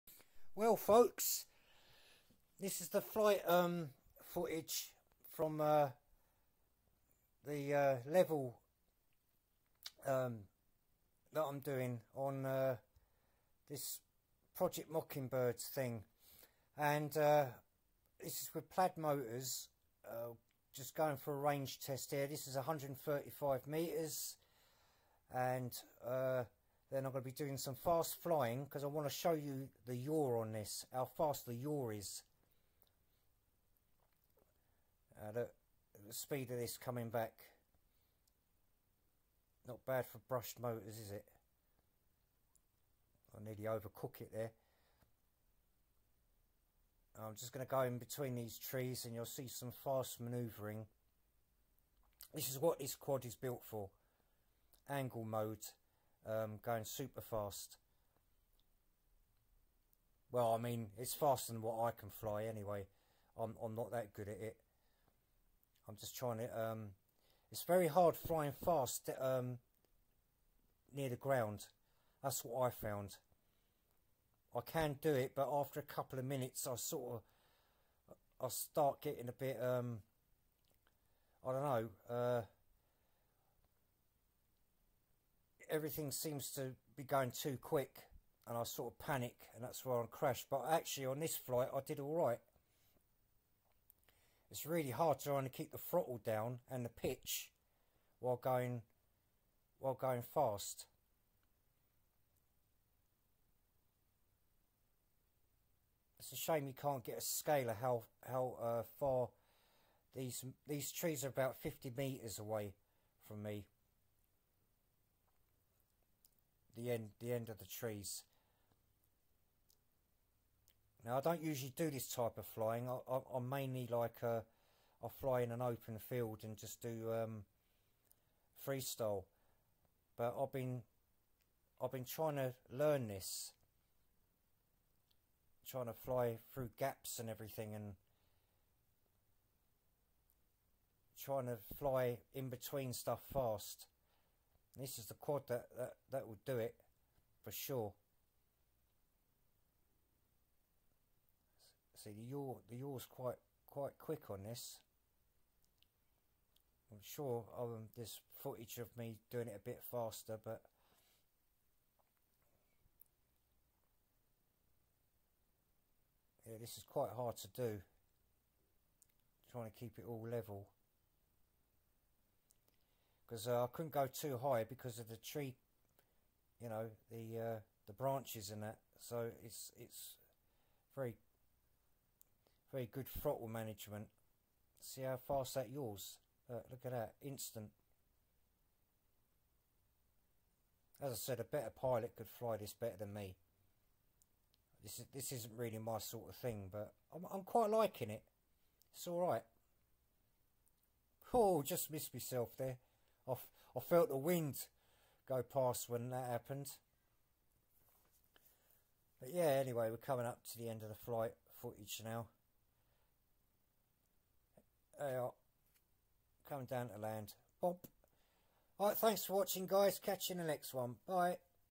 NEWBEEDRONE HUMMINGBIRD V1 PROJECT MOCKINGBIRD EDITION FAST FLY
Not my normal flying style but I'm quite getting into this race thing!I'm using the Betafpv lite radio 2 for the transmitter and it's perfect for the job,many top racers use this transmitter too.